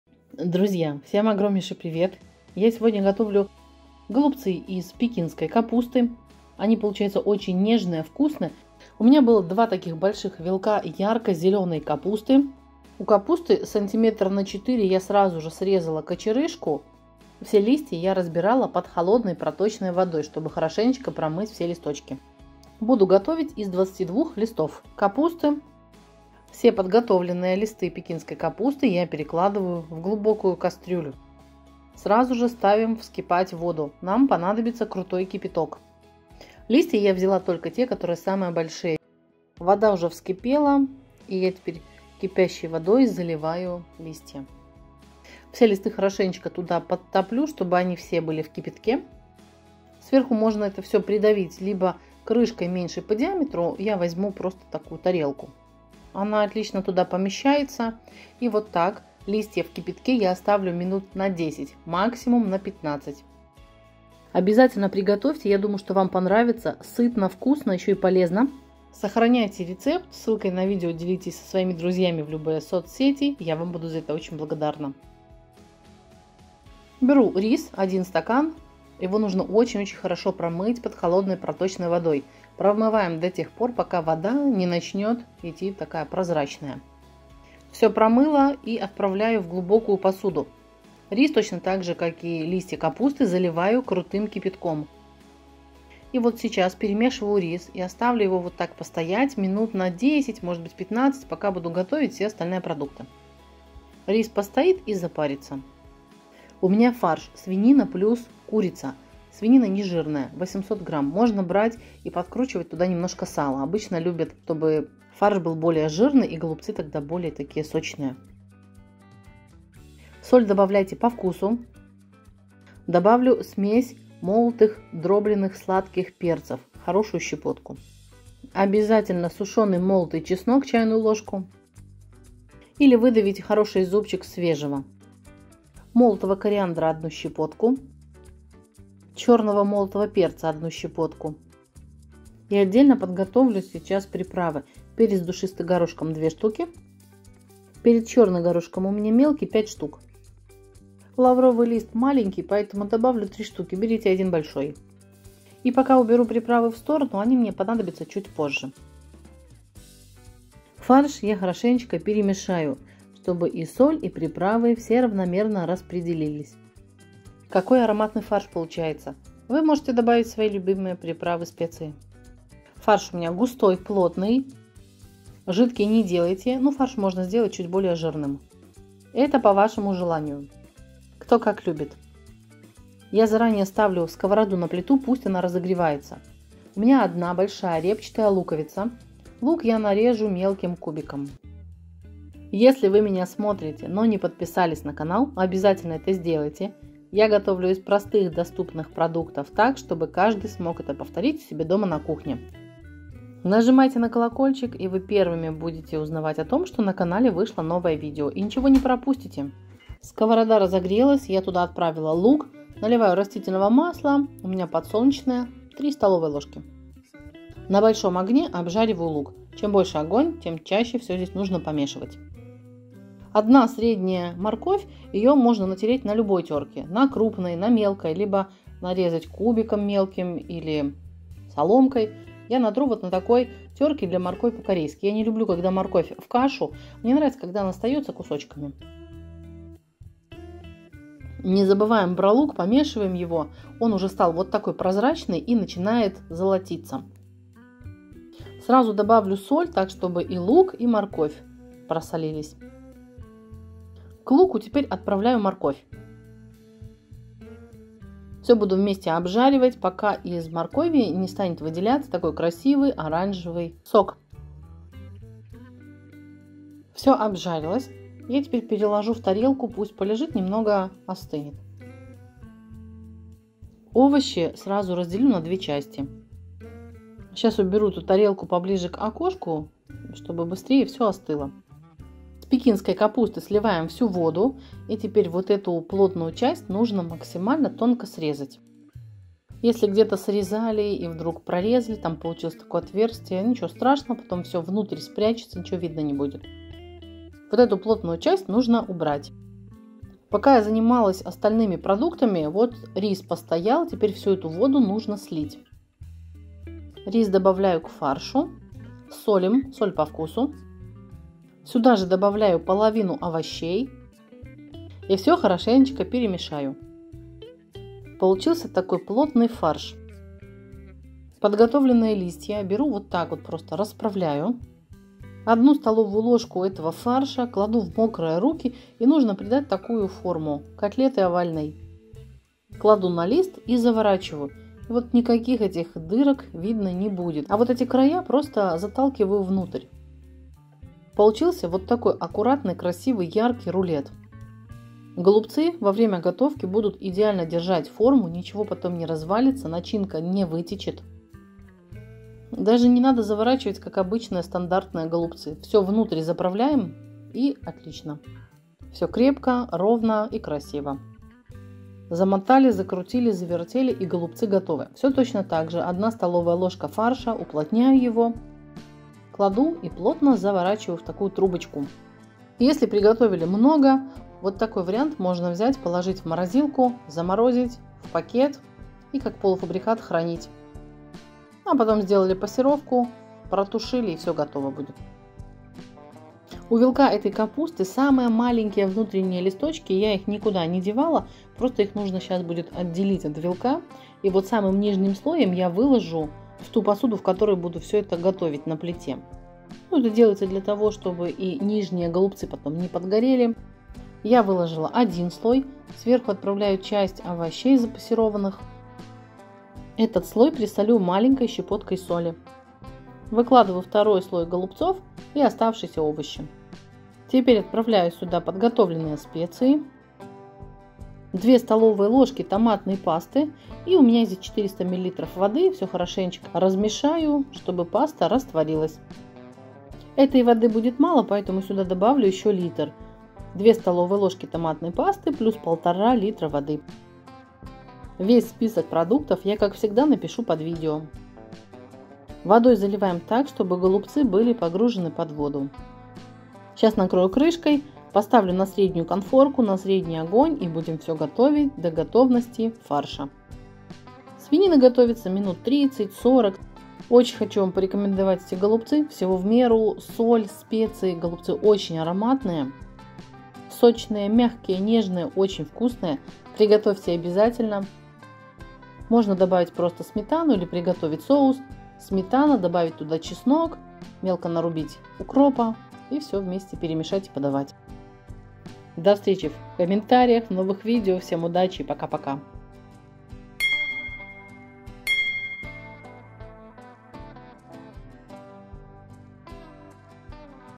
САМЫЕ НЕЖНЫЕ и вкусные ГОЛУБЦЫ из пекинской капусты!
Ингредиенты:
Листья пекинской капусты - 22 штуки
Рис - 1 стакан
Кипяток - 3 литра
Фарш свинина + курица - 800 грамм
Соль по вкусу
Смесь сладких молотых перцев - 1 щепотка
Молотый кориандр - 1 щепотка
Молотый сушёный чеснок - 1 чайная ложка
Чёрный молотый перец - 1 щепотка
Перец душистый горошком - 2 штуки
Перец чёрный горошком - 5 штук
Лавровый лист - 1 штука
Растительное масло - 3 столовые ложки
Репчатый лук - 1 штука большая
Морковь - 1 штука средняя
Томатная паста - 2 столовые ложки + вода - 1,5 литра
Для подачи сметана